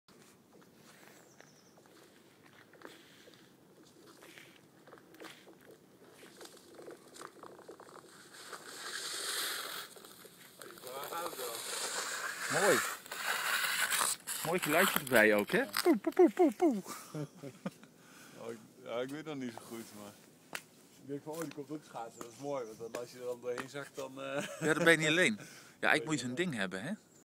Eerste schaatser Lonnekermeer 2012
2 februari 2012 om 10 uur testte deze schaatser het ijs van ca 8 cm dik. Ging prima, maar toch wel wat "zoemend" ijs. Tóch nog maar een of twee nachten wachten...